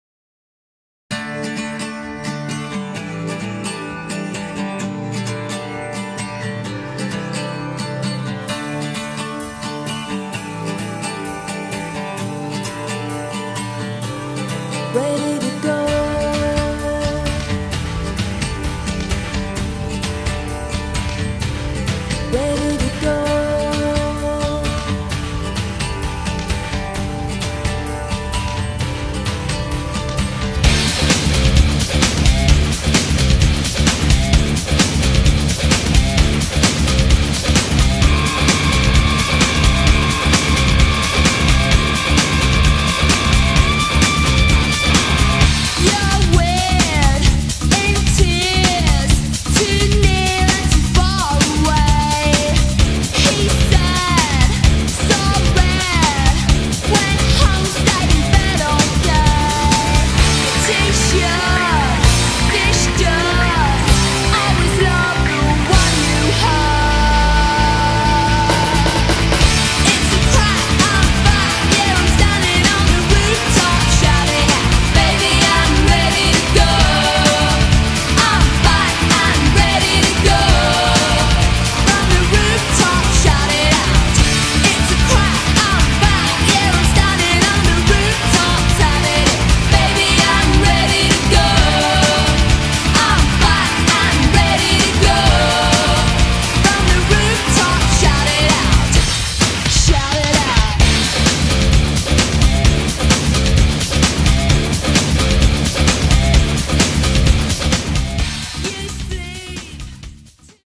Saloon Stock Car World Championship Weekend Preview
Preview package of the forthcoming Saloon Stock Cars World Championship weekend at Smeatharpe Stadium on Saturday 11th and Sunday 12th August 2012.

Also in action Saloon Stock Cars supporting programme, BriSCA F2 Stock Cars and National Bangers.